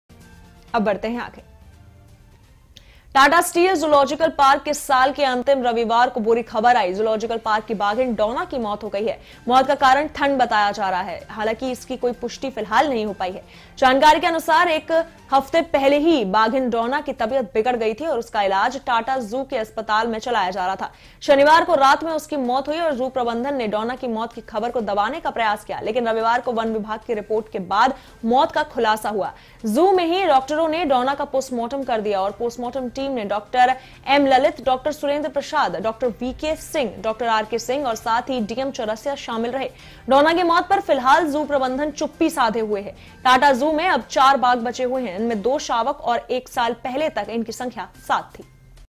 Jamshedpur : TATA Steel Zoological Park में बाघिन डोना की मौत, ठंड की आशंका
1. Begusarai: केंद्रीय मंत्री गिरिराज सिंह का बयान, देश का माहौल खराब कर रहा विपक्ष, मुसलमानों को उकसा रहे ओवैसी
2. प्रशांत किशोर का बड़ा बयान- बिहार में BJP नही JDU बड़ी पार्टी, सियासत में उबाल (Nalanda)
3. ठंड से ठिठुरा Bihar, Gaya में तापमान 2.4 डिग्री सेल्सियस तक पहुंचा
4. Bhagalpur में बदमाशों ने नौ साल की बच्ची को मारी गोली, JLNMCH में भर्ती
5. Muzaffarpur में पेट्रोल पंप से लूट, कोहरे का फायदा उठा 24 लाख लूट ले गए बदमाश
6. Jharkhand में ठंड से 3 दिन में 15 की मौत, 2 जनवरी तक ऐसा ही रहेगा मौसम (Dhanbad)
7. Ramgarh: अड़की में पुलिस और नक्सलियों के बीच मुठभेड़, सर्च ऑपरेशन जारी
8. TATA Steel Zoological Park  में बाघिन डोना की मौत, ठंड की आशंका (Jamshedpur)
9. Deoghar: नववर्ष पर देवघर मंदिर के साज-सज्जा की तैयारी पूरी, प्रशासन ने कसी कमर